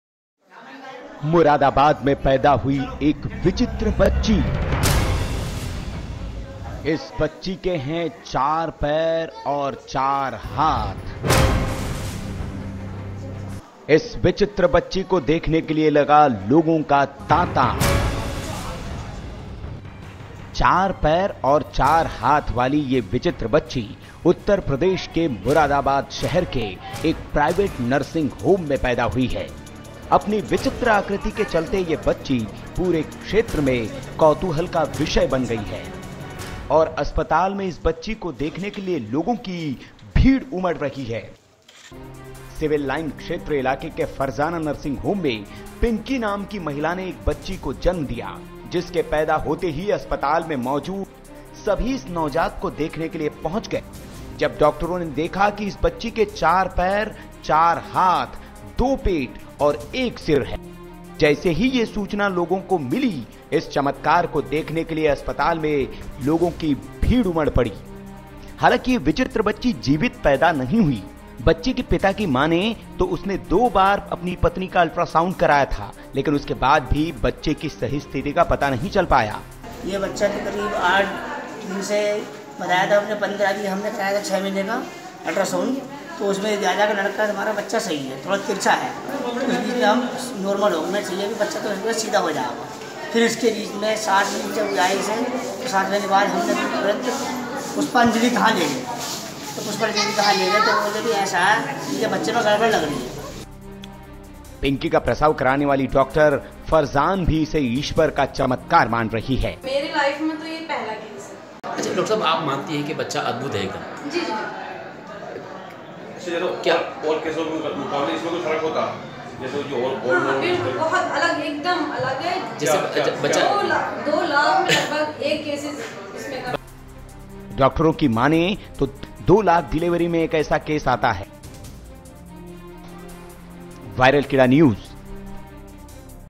चमत्कार- पैदा हुई चार हाथ और पैर वाली बच्ची | Strange News Video
Uttar Pradesh के Moradabad के एक निजी नर्सिंग होम में पैदा हुए बच्चे ने पूरे इलाके में कौतूहल बढ़ा दिया। इस बच्ची के चार हाथ, चार पैर और एक सिर देखकर लोग कहने लगे कि यह तो देवता की तरह लग रहा है। ये Strange News सुनकर बच्ची को देखने के लिए लोग उमड़ पड़े।
Strange news from UP Moradabad- child born with four feet, four hand and one head.

tweeter-
Google+-